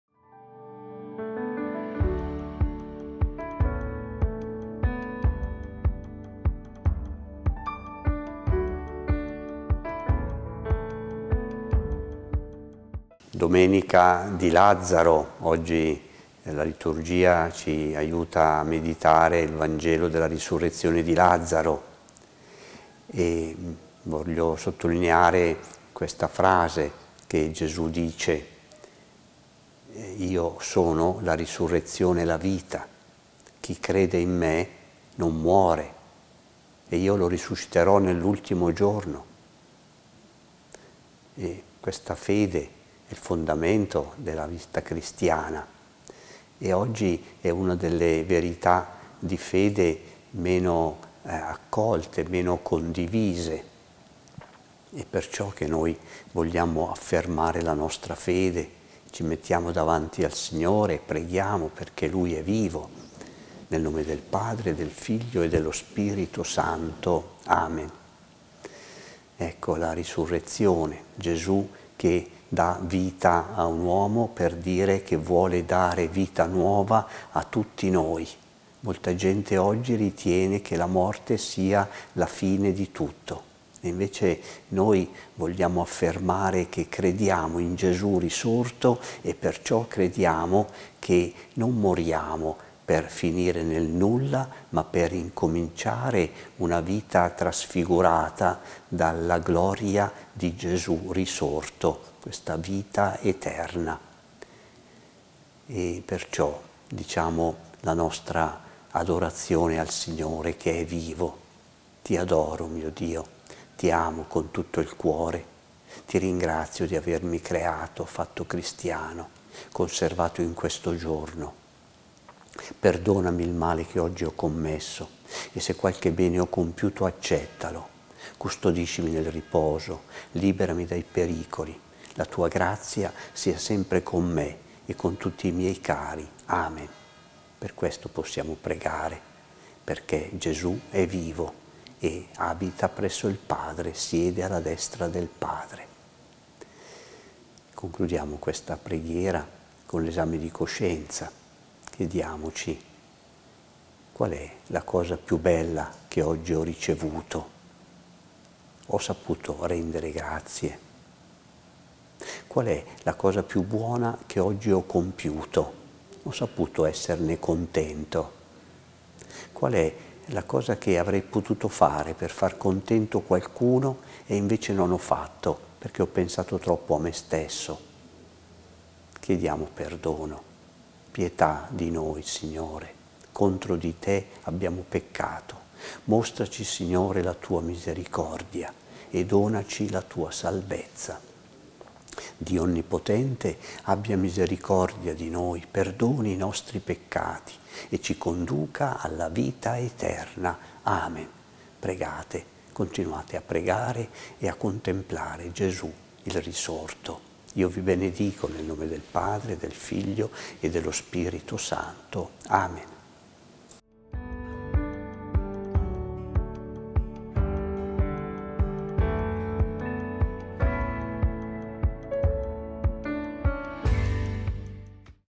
“Credo”: domenica 17 marzo, V settimana di Quaresima
«Gesù risorto vuole dare vita nuova a tutti noi»

Dal Battistero di Castiglione Olona (VA)